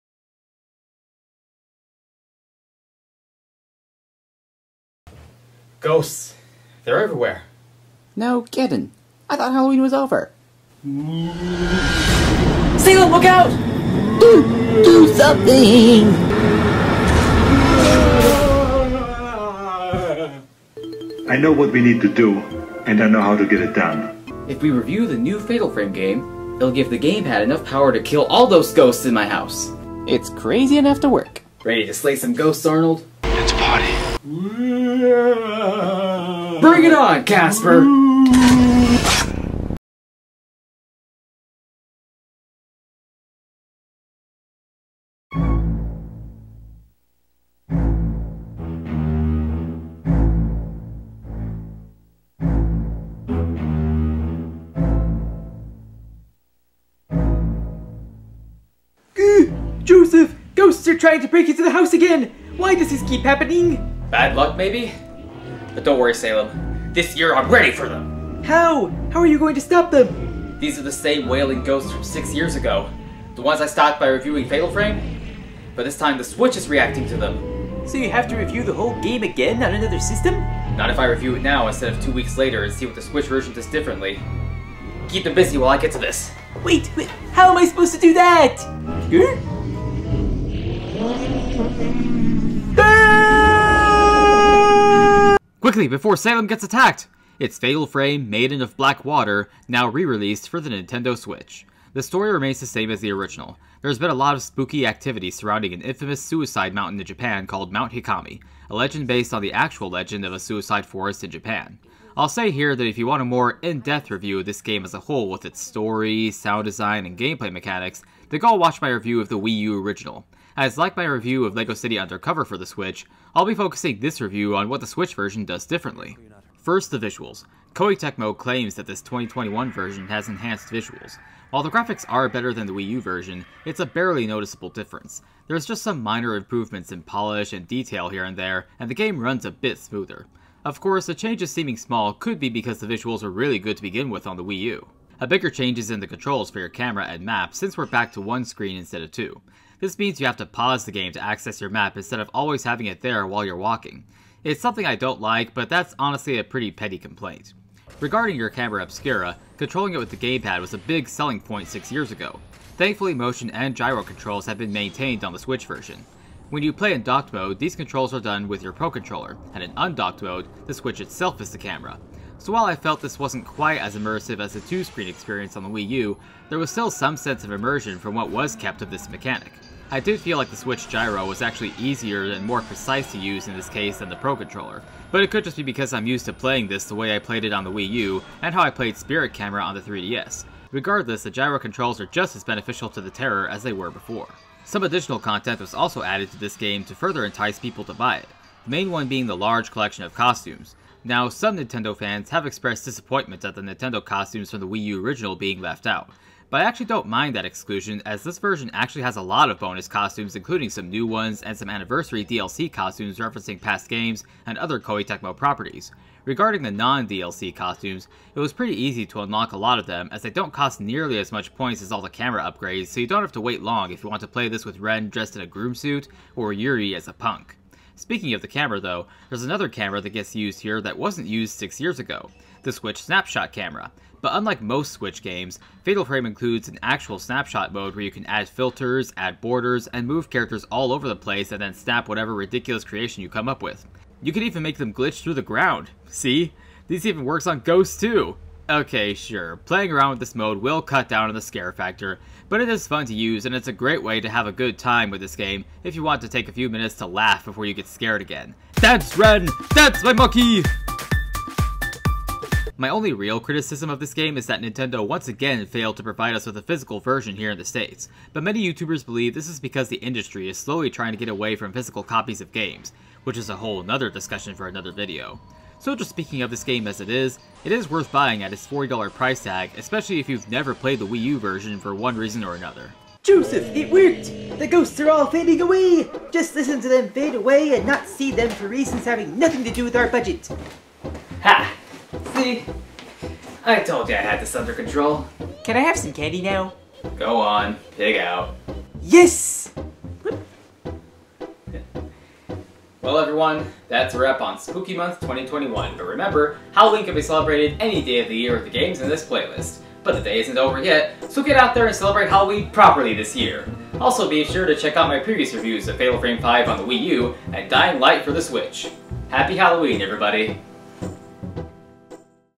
Fatal Frame Maiden of Black Water (Switch) Review
The Nintendo Reviewer fights off ghosts from Halloween past in Fatal Frame Maiden of Black Water for the Nintendo Switch.